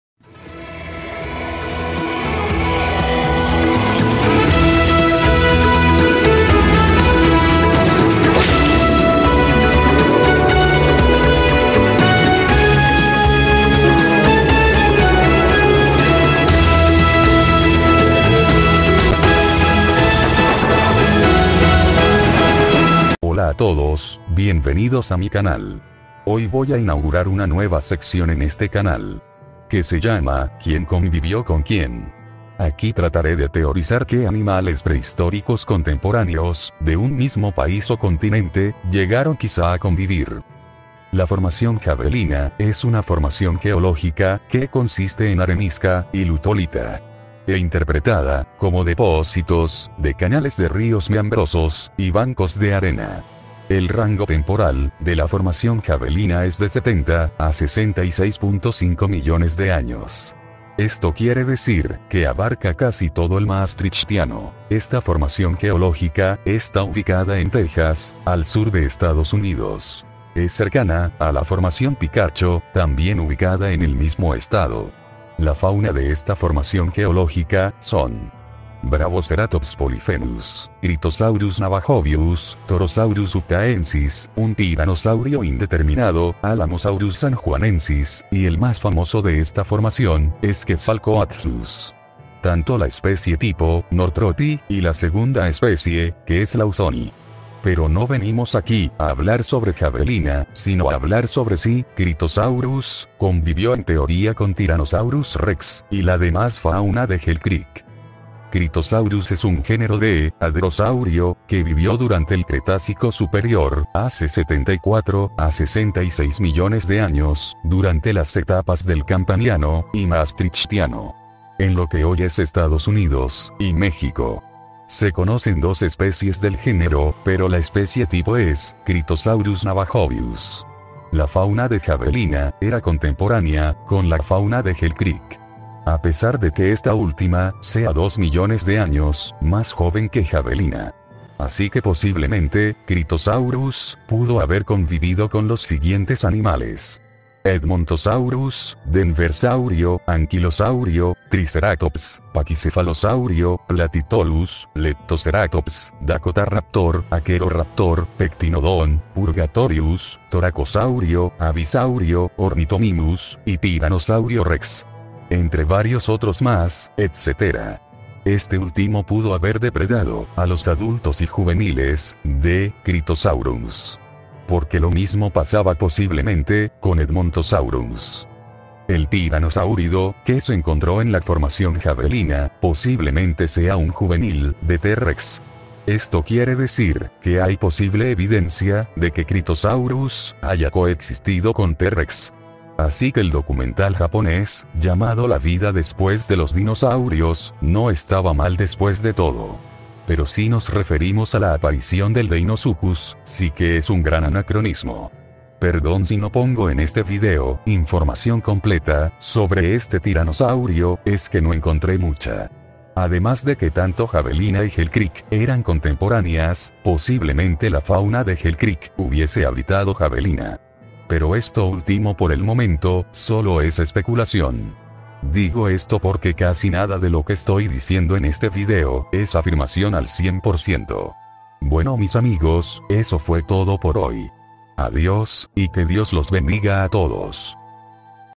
Quien Convivió con quien Parte 1 - (T-Rex y Kritosaurus) [Teoria]
La verdad es que dije que Casi todo aquí es Especulación Exeptuando, Las Partes donde digo que hay Evidencia de que El Tyranosaurus Rex Vivió en Javelina, y Los datos básicos del kritosaurus y los Datos Básicos de las Formaciones geológicas y cual Paleo fauna vivió allí.